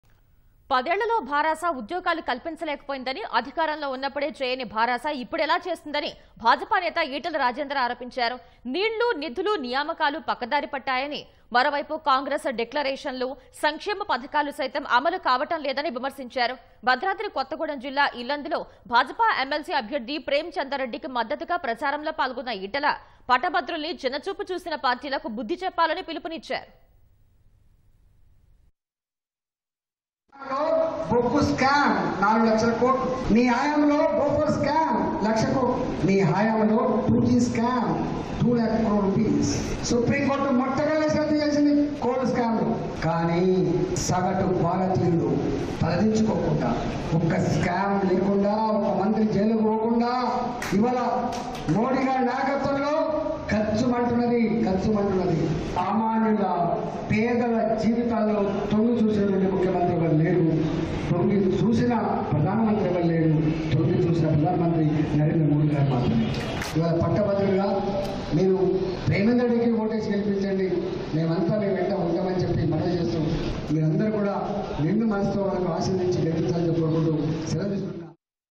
BRS Govt Not Fulfilled Jobs | Etela Rajender Alleged | పదేళ్లలో భారాస ఉద్యోగాలు కల్పించలేకపోయింది!
పదేళ్లలో భారాస ఉద్యోగాలు కల్పించలేకపోయిందని అధికారంలో ఉన్నప్పుడే చేయని భారాస ఇప్పుడెలా చేస్తుందని భాజపా నేత ఈటల రాజేందర్  ఆరోపించారు. నీళ్లు, నిధులు, నియామకాలు పక్కదారి పట్టాయని మరోవైపు కాంగ్రెస్  డిక్లరేషన్లు, సంక్షేమ పథకాలు సైతం అమలు కావడం లేదని విమర్శించారు. భద్రాద్రి కొత్తగూడెం జిల్లా, ఇల్లందులో భాజపా ఎమ్మెల్సీ అభ్యర్థి ప్రేమ్ చందర్  రెడ్డికి మద్దతుగా ప్రచారంలో పాల్గొన్న ఈటల... పట్టభద్రులను చిన్నచూపు చూసిన పార్టీలకు బుద్ధి చెప్పాలని పిలుపునిచ్చారు.